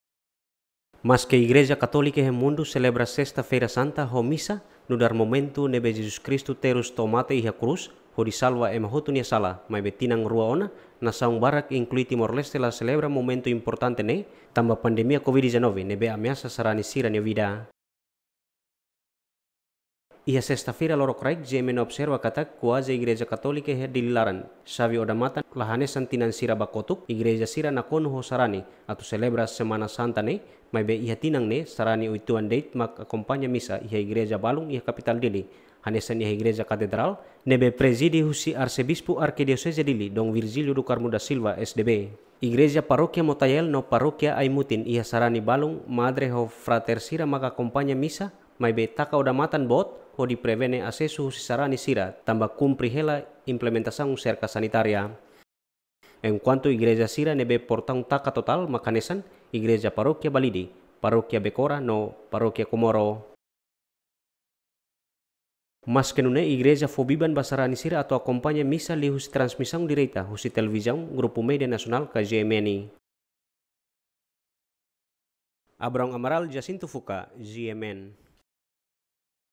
Sarani la partisipa misa selebrasaun Sesta Feira Santa tanba kumpri regrás Serka Sanitária
Sarani sira la partisipa misa selebrasaun Sesta Feira Santa iha Igreja sira tanba kumpri regrás Serka Sanitária